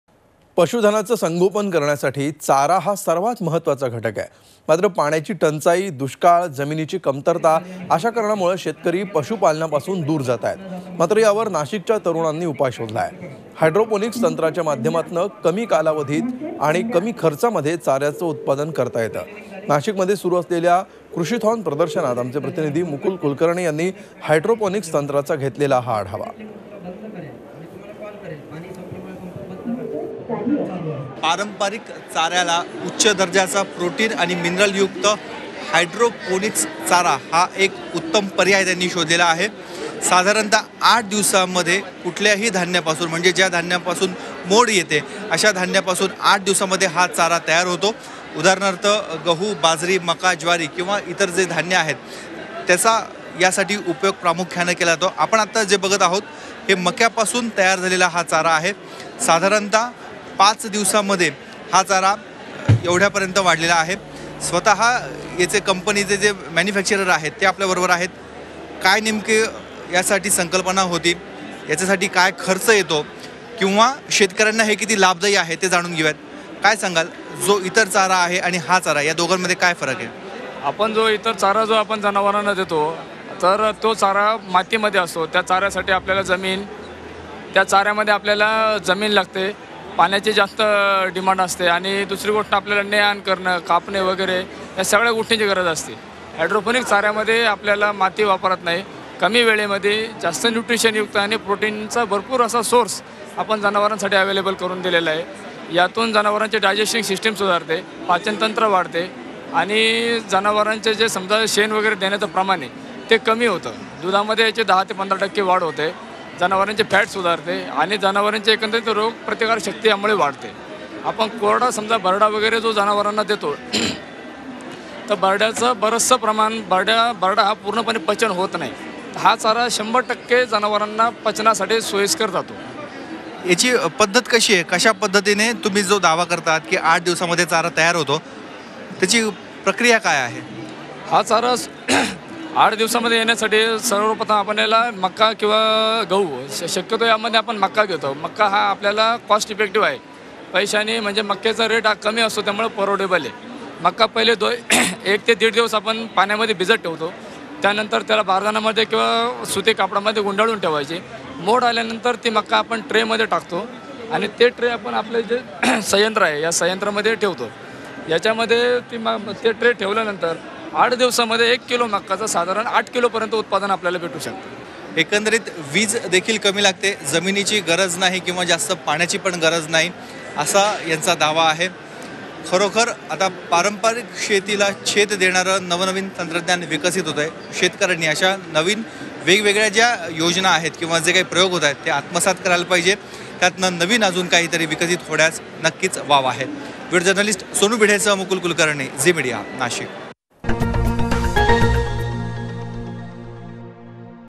पीक पाणी । गुरांच्या चा-यासाठी हायड्रोपोनिक्स नवं तंत्र फायद्याचं
Nashik Peekpani On Hydroponics Tech Useful For Animals Food